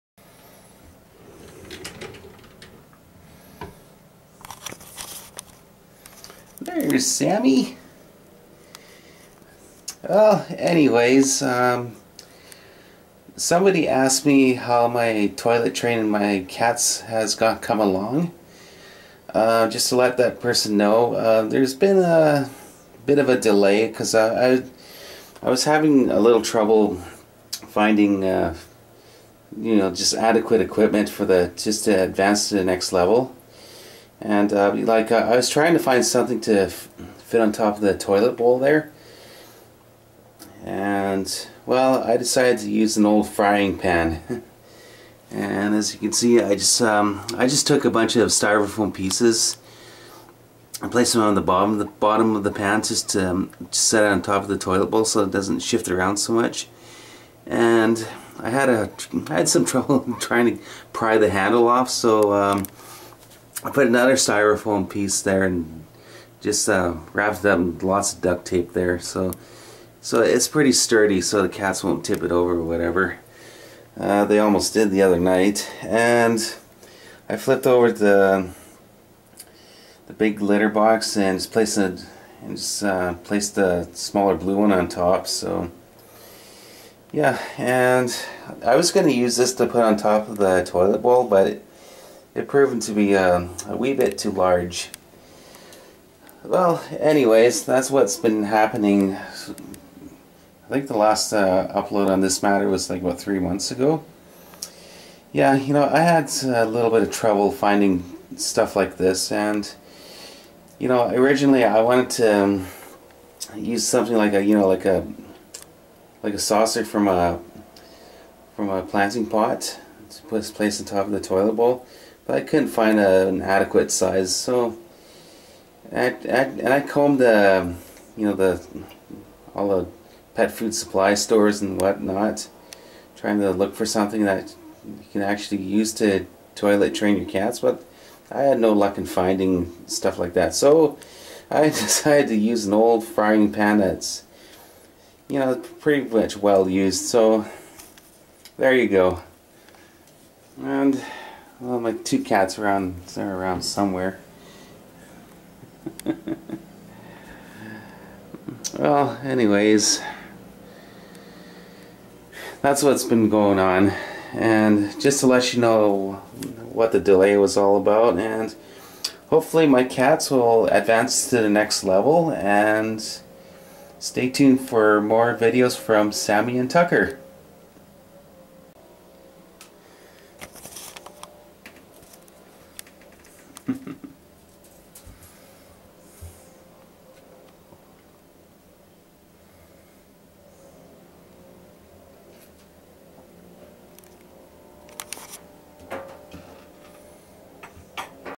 Toilet Training My Cats: Part 4
Still trying to toilet train my 2 cats. I haven't given up yet.